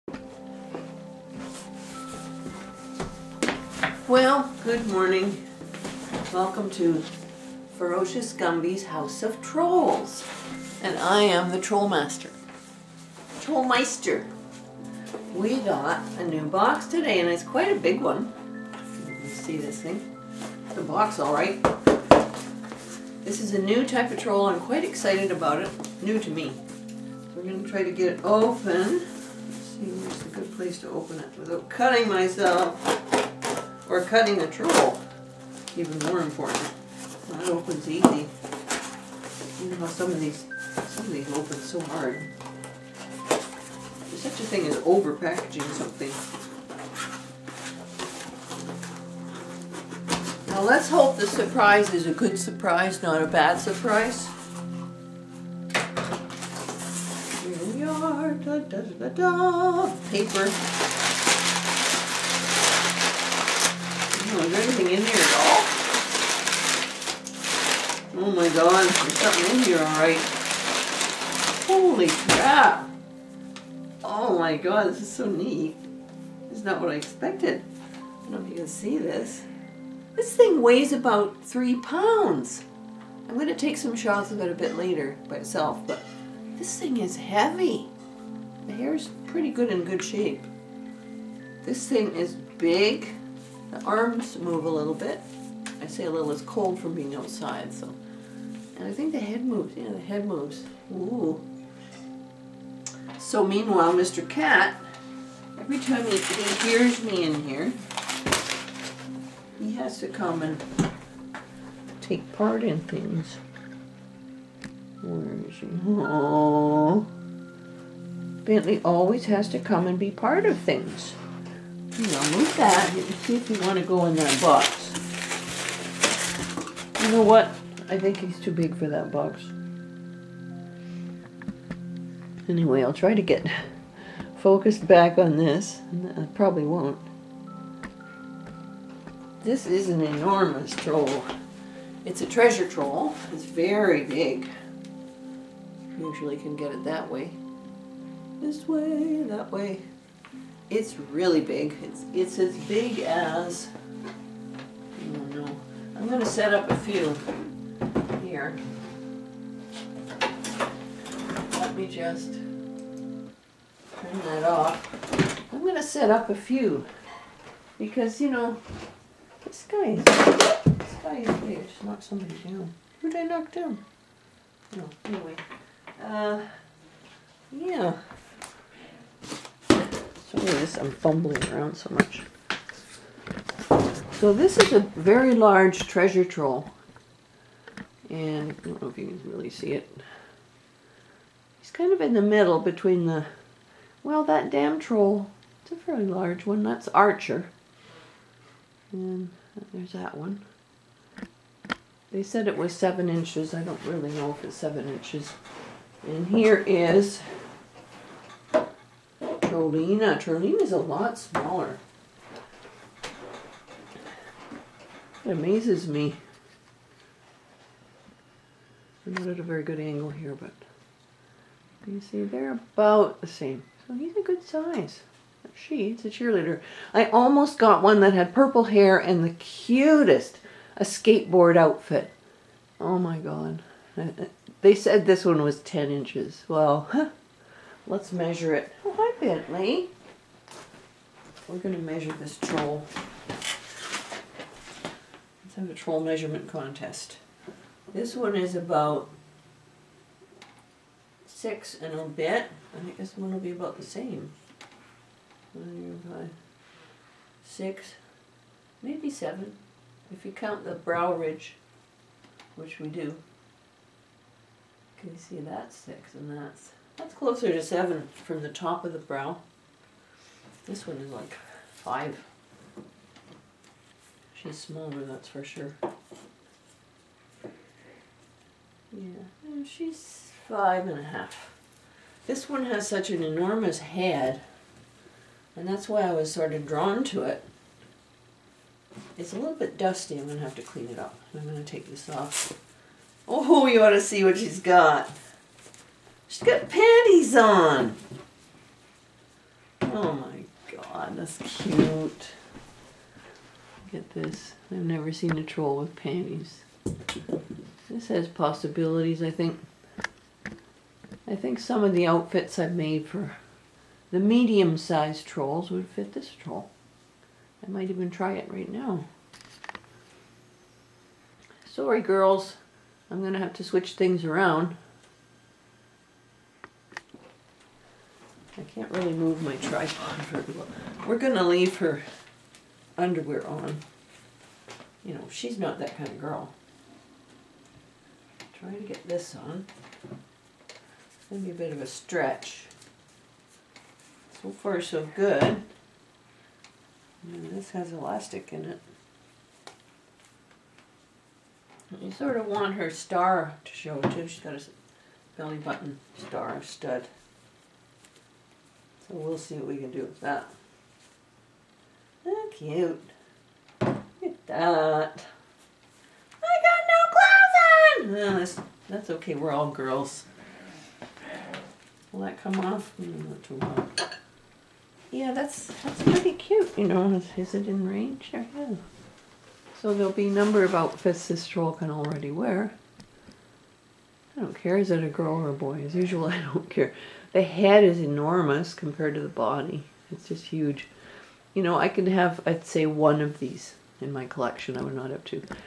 The Troll Doll Channel: Box Opening for HUGE Treasure Troll!
My new Treasure Troll might be the heaviest troll I own, perhaps because of the size and weight of its head. Though it's cute, it creeps me out a bit because the head/eyes are so big! But it's nice to have one in my collection. I still prefer normal-sized Treasure Trolls, which are in better proportion. (Note: this ran too long, so I edited out 3 minutes at the end, which is why it ends so abruptly.)